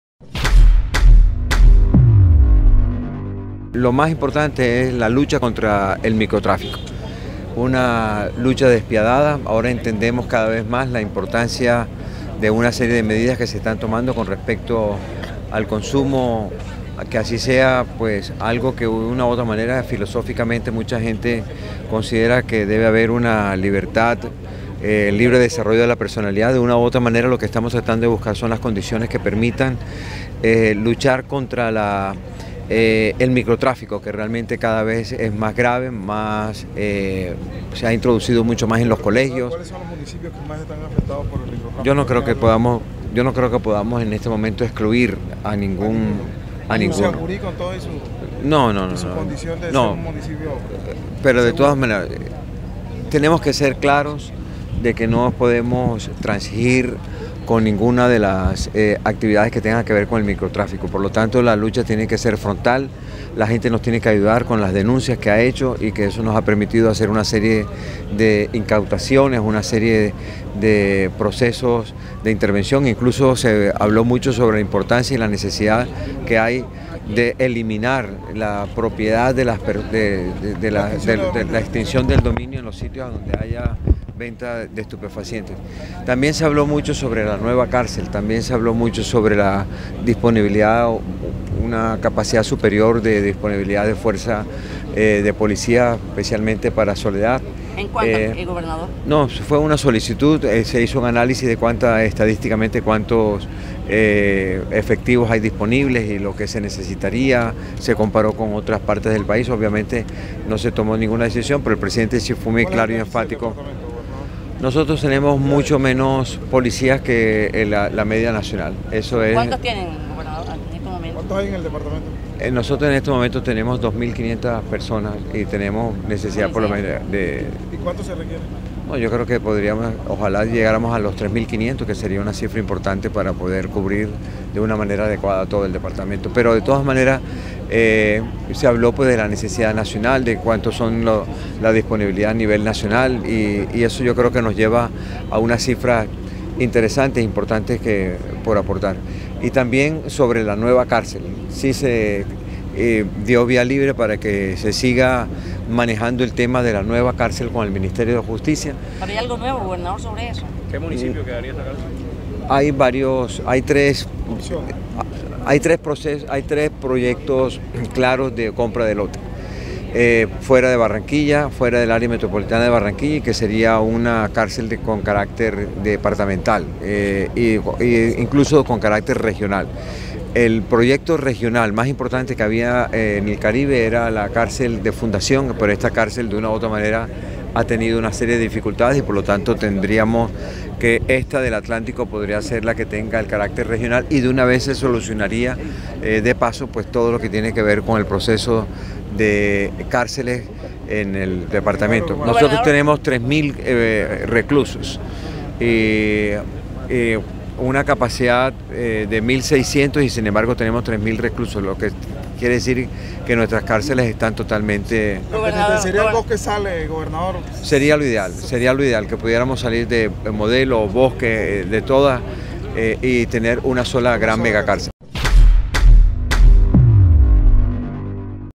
"Ningún municipio del Atlántico se salva del microtráfico", se lamenta Verano
El gobernador Eduardo Verano De la Rosa fue enfático al afirmar este viernes, tras el consejo de seguridad con el presidente Duque y la cúpula de las Fuerzas Armadas y la Policía, que en estos momentos ningún municipio del Atlántico puede ser excluido de las cifras del microtráfico, pues esta problemática ha alcanzado hasta los colegios.

"Es una lucha despiadada y que ahora entendemos la serie de medidas que ha tomado el gobierno nacional", dijo el mandatario departamental.

Agregó que cada vez es más grave la situación del microtráfico y se ha adentrado en las instituciones.

"La lucha tiene que ser frontal, pues ha permitido todas estas capturas que se han producido en las últimas semanas, así como la incautaciones de la Policía", sostuvo.

El gobernador explicó que las incautaciones y procesos de intervención son necesarias, por eso la importancia y la necesidad de aplicar la ley de extinción de dominio donde se registre venta de estupefacientes.

Proyecto de megacárcel regional
El gobernador del Atlántico sostuvo que uno de los puntos manejados en el Consejo de Seguridad en la Escuela Antonio Nariño con el presidente Iván Duque fue el de la construcción de la megacárcel regional.

"Hay tres proyectos claros de compra de lotes fuera de Barranquilla. Sería una cárcel con carácter departamental o regional", aseveró.

Añadió que este megaproyecto solucionaría el problema de hacinamiento "y lo ideal sería salir de El Bosque y La Modelo".